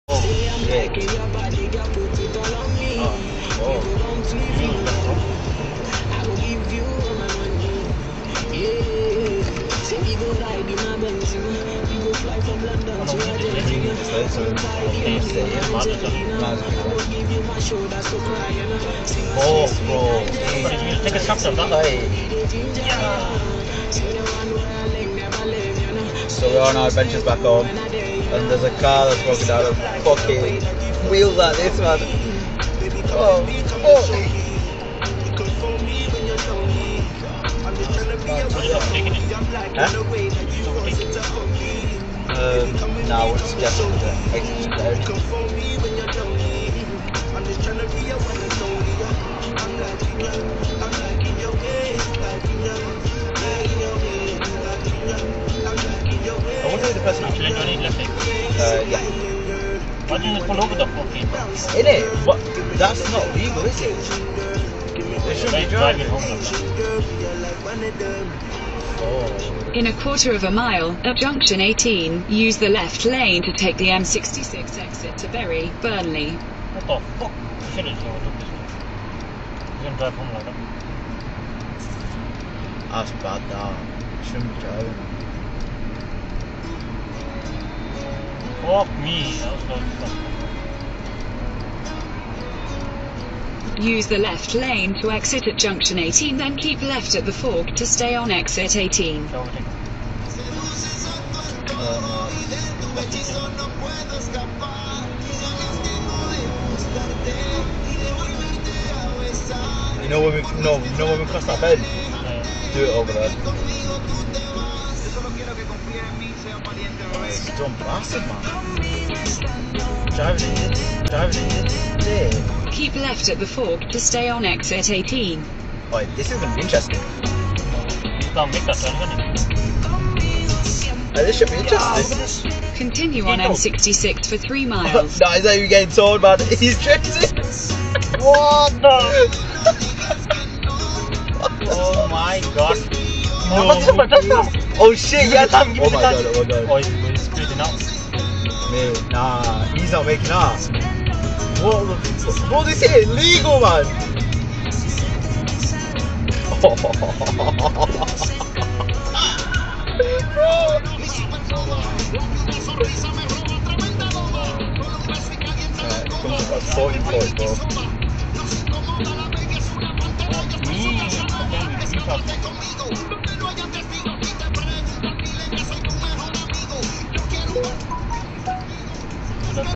Dangerous Broken Down Bent Wheels Car Keeps Driving
In this video I show you a car that keeps on driving dangerously with limited to no steering power due to his axles snapping and his wheels were fully bent inwards on the motorway at 70mph and then he drove at 40mph the whole way instead of pulling over and getting recovery truck.